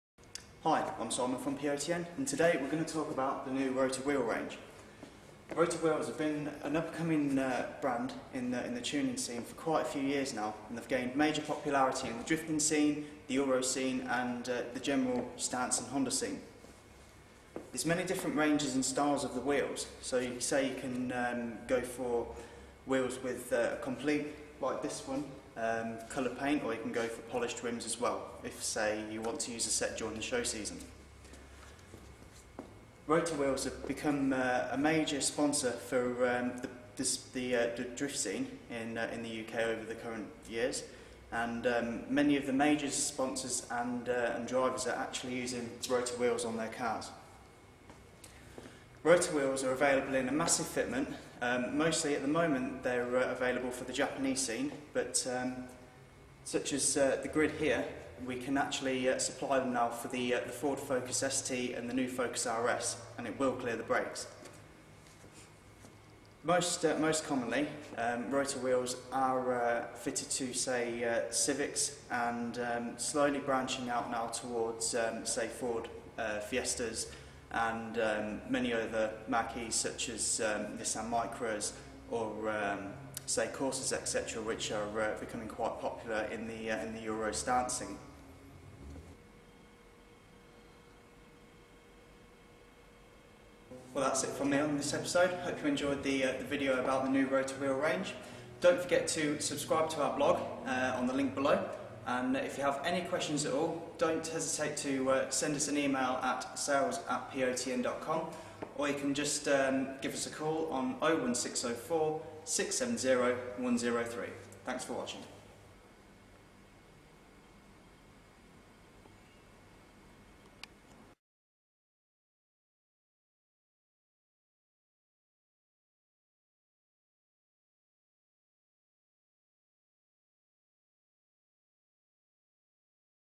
Short Explanation of Rota Wheels.
Model specific videos which will offer in depth offsets and fitments coming soon!